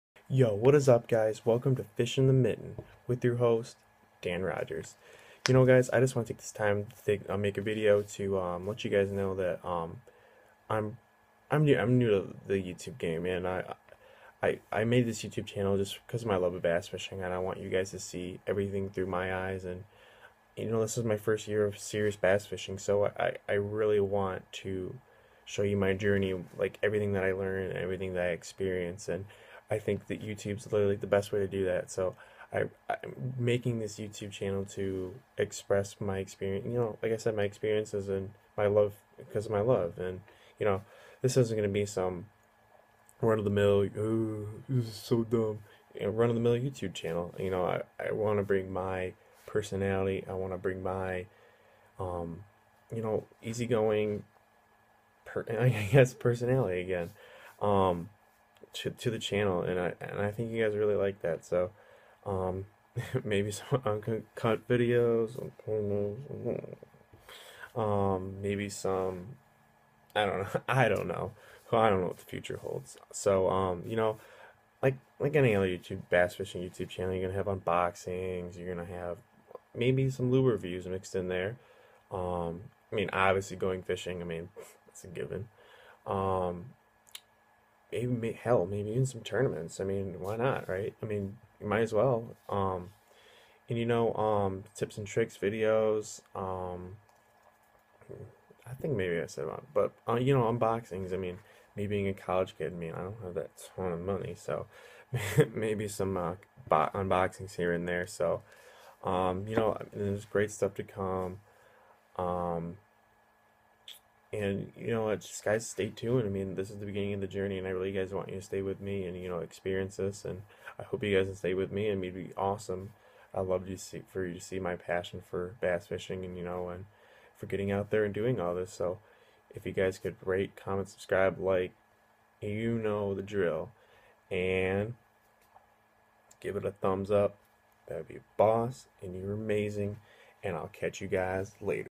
Welcome to Fishin' the Mitten!!!
Hey guys I'm coming at you with my first YouTube vid. I hope you liked it!! This is only the start there is no telling where this channel can go and the heights it can achieve.

PS: Sorry for the bad quality vid. I'm trying to work out a long term video taking station.